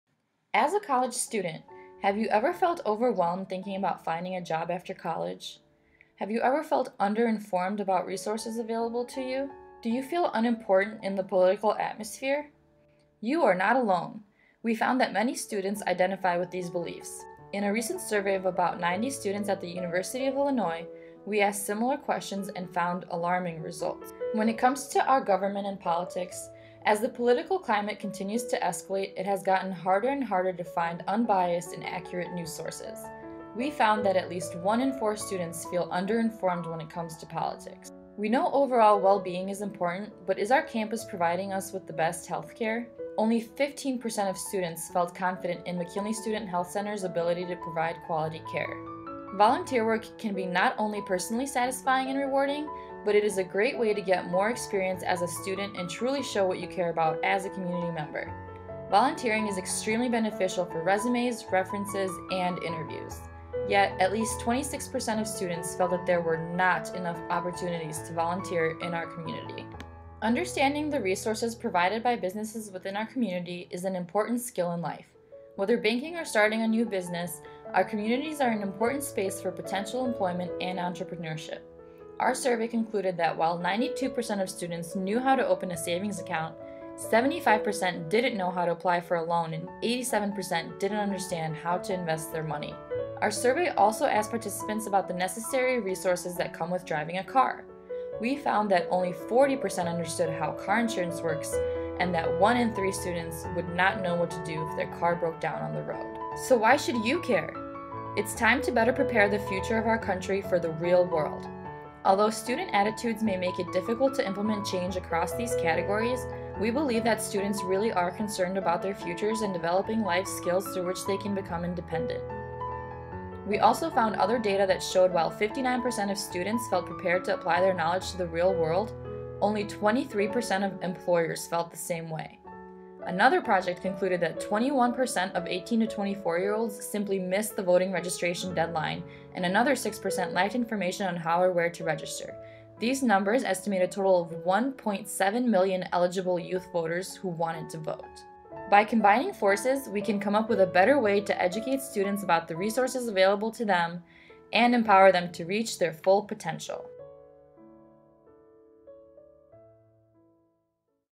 AGED 480 Life Skills Stage 1 Video
○ We believe a resource fair would be a great alternative to workshops and lectures we have experienced as college students. By having a designated time and place where we could go to have our important questions answered, we would have the ability to learn about the multitude of resources available within the campus community as well as helpful life skills. We invite you to contribute to an experience that will positively impact the University of Illinois Champaign-Urbana community and help make our students more knowledgeable of what is available to them.